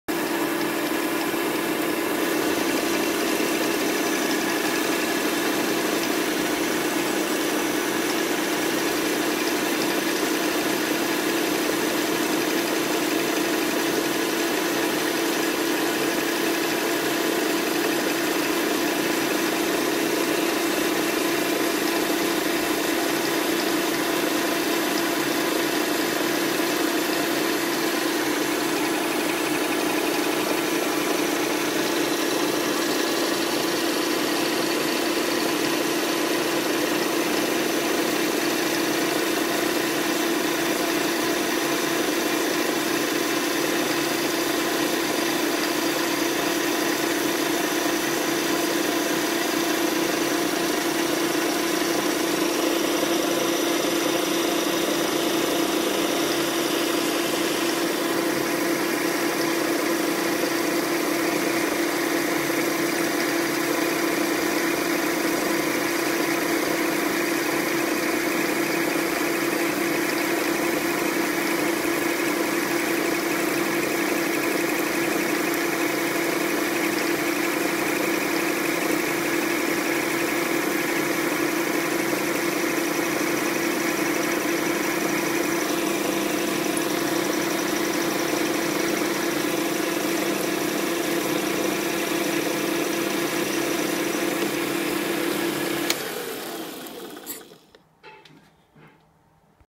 Точение длинного конуса. Turning of a long cone
Точение длинного конуса смещая ось расточной головкой с вращающимся центром.